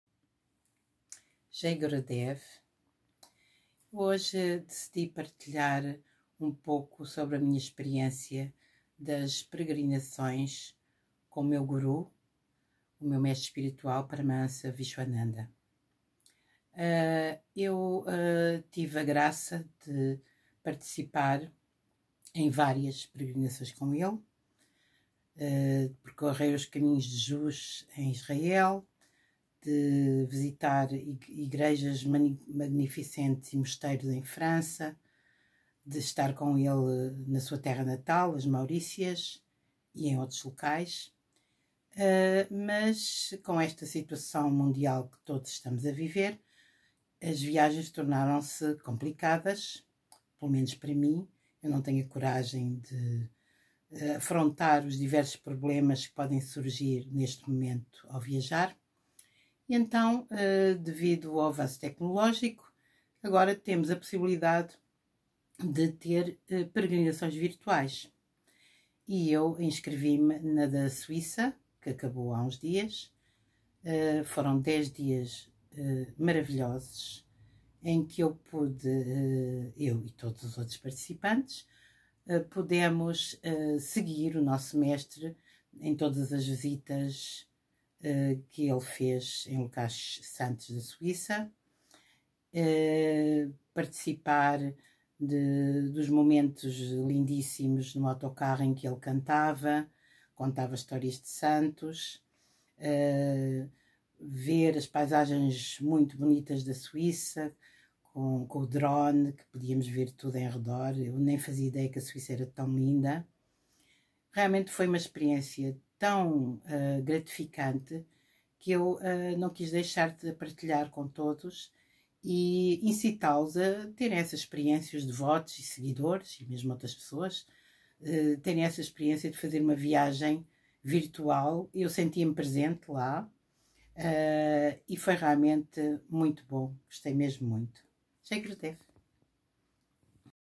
Peregrinações virtuais com Paramahamsa Vishwananda /Virtual Pilgrimages with Paramahamsa Vishwananda
Neste curto vídeo falo um pouco sobre a minha experiência na 1a peregrinação virtual com o Guruji à Suíça em Maio de 2021.